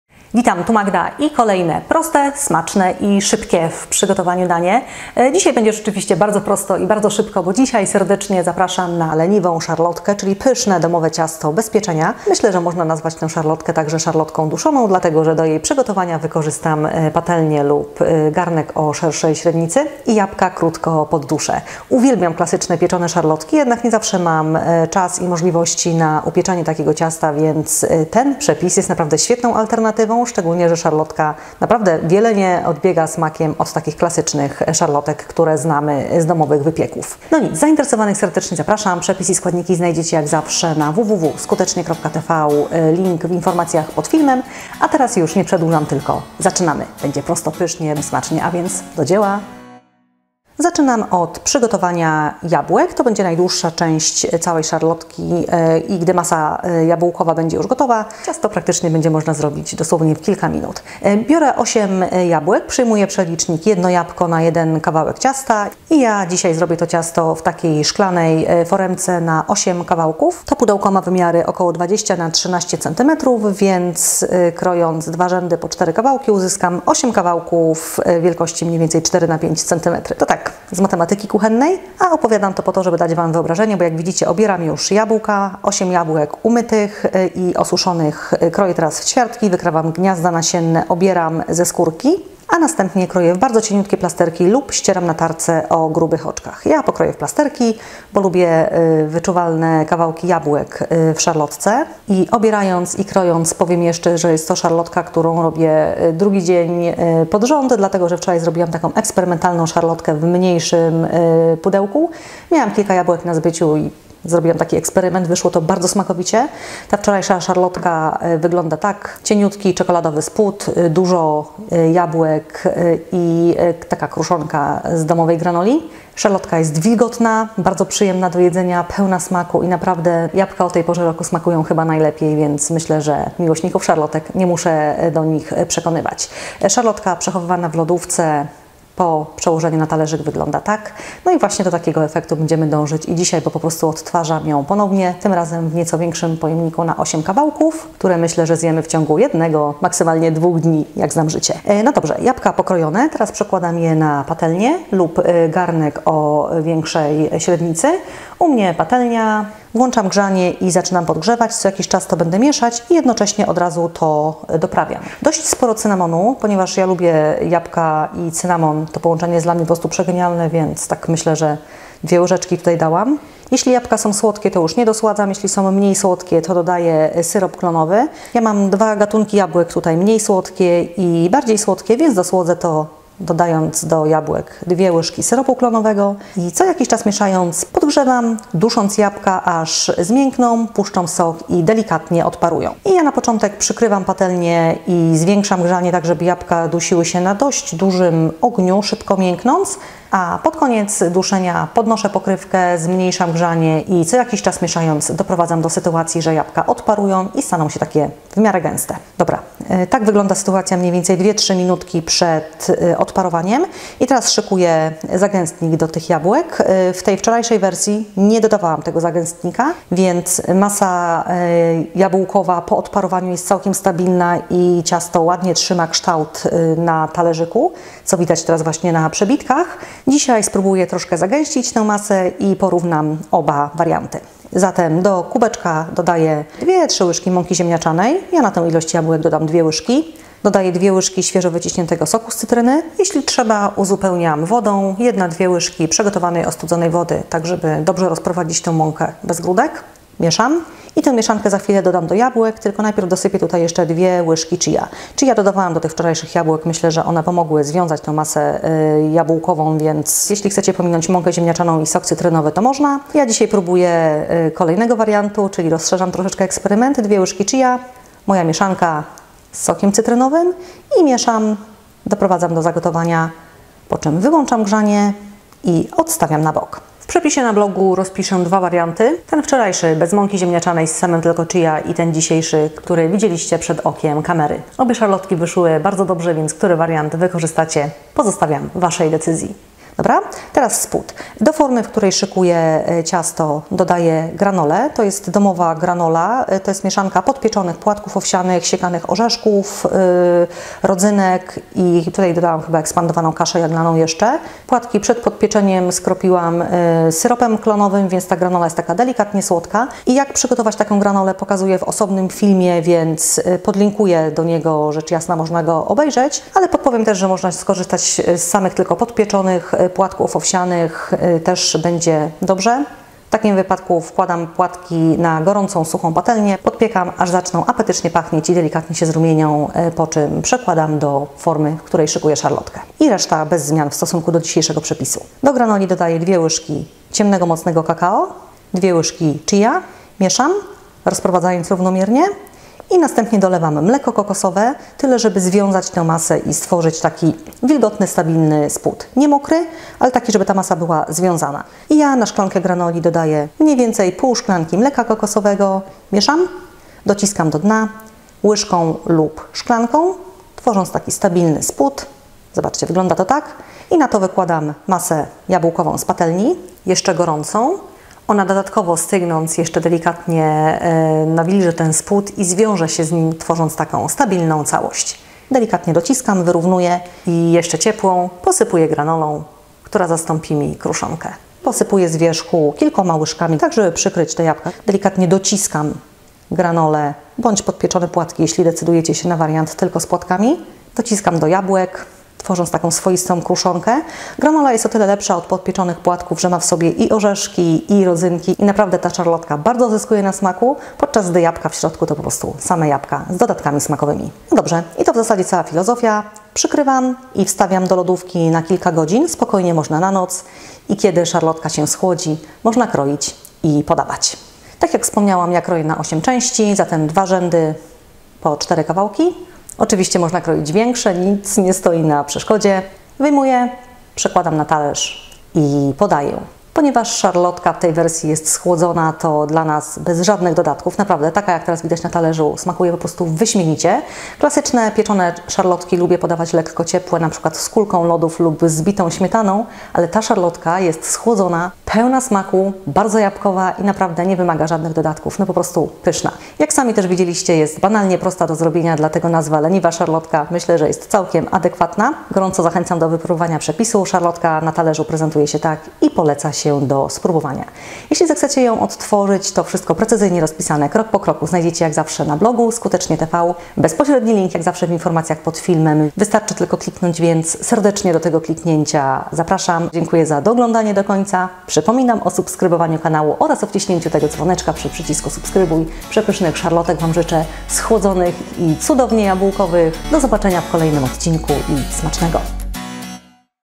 Leniwa szarlotka (BEZ PIECZENIA) :: Skutecznie.Tv
Leniwa SZARLOTKA, czyli przepis na domową szarlotkę: prostą, szybką i bez pieczenia! Super do kawy, herbaty, w roli przekąski... a nawet śniadania, bo jest nie tylko lekko słodka i pyszna, ale także wartościowa odżywczo. Kto się skusi? ;)

TOP 20